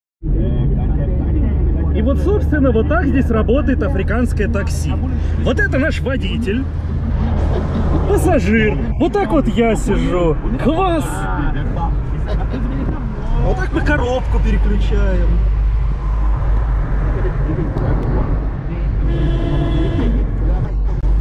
African taxi in Togo
African taxi in Togo (Lome). Did you know that african taxis are 5-6 seaters plus driver? That was some fun experience for me that I've encountered at Lome, the capital of Togo in Western Africa region. I thought that I've secured myself a comfort ride by sitting in the forward passenger seat. Hell yeah. Imagine my face, when after a two minute ride the driver stopped to pick up another passenger and invited her to my seat :) Bthw, the ride was only 15 cents.